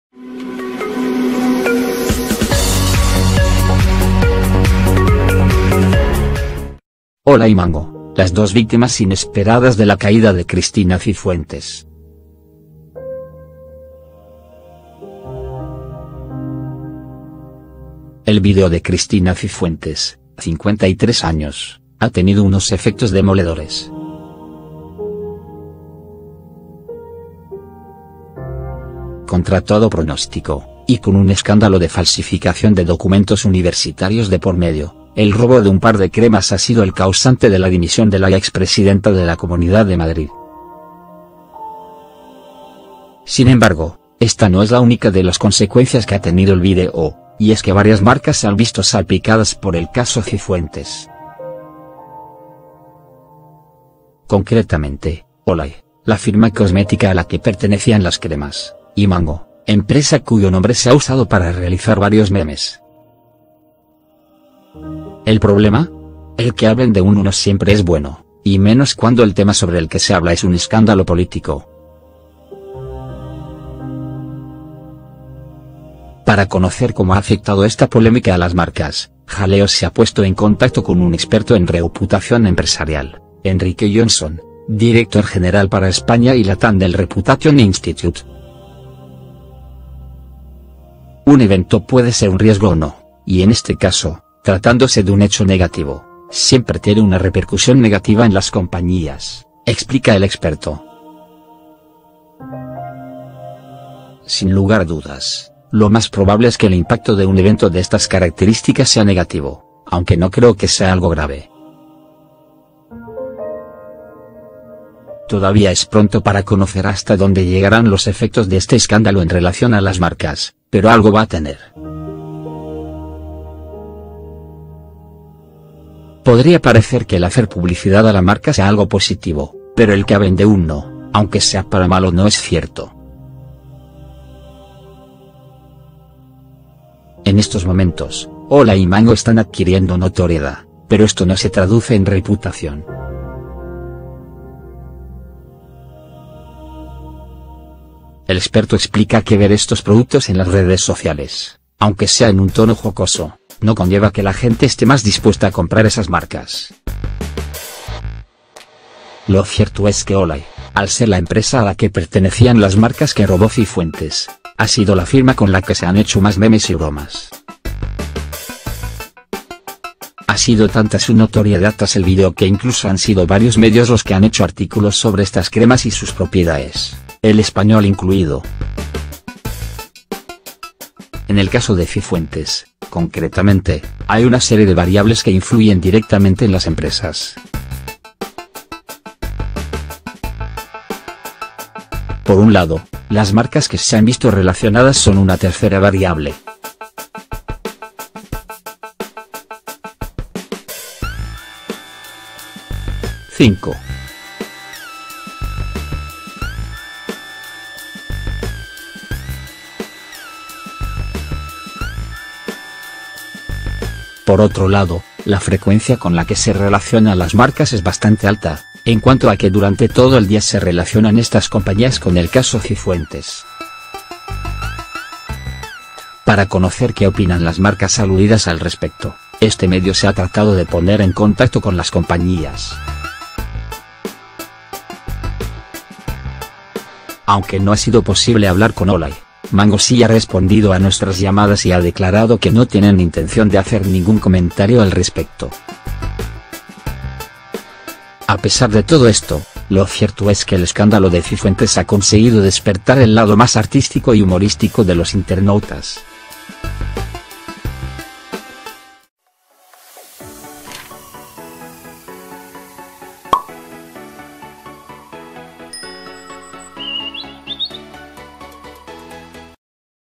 Olay y Mango, las dos víctimas inesperadas de la caída de Cristina Cifuentes || LVTNAszok
El vídeo de Cristina Cifuentes (53 años) ha tenido unos efectos demoledores. Contra todo pronóstico, y con un escándalo de falsificación de documentos universitarios de por medio, el robo de un par de cremas ha sido el causante de la dimisión de la ya expresidenta de la Comunidad de Madrid.
►® Si t